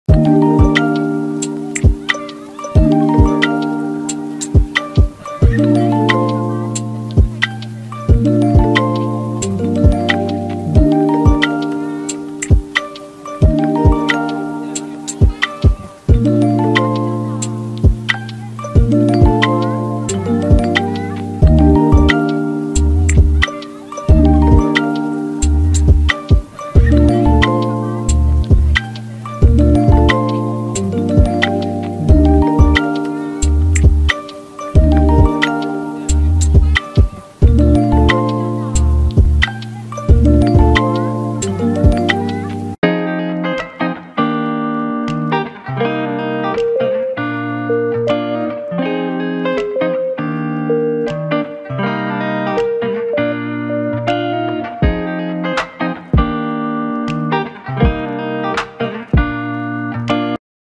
8视界娱乐生活志｜父子档携手卖怡保咖喱面 延续祖母曾祖母的老味道
这碗咖喱面，就是有家乡味！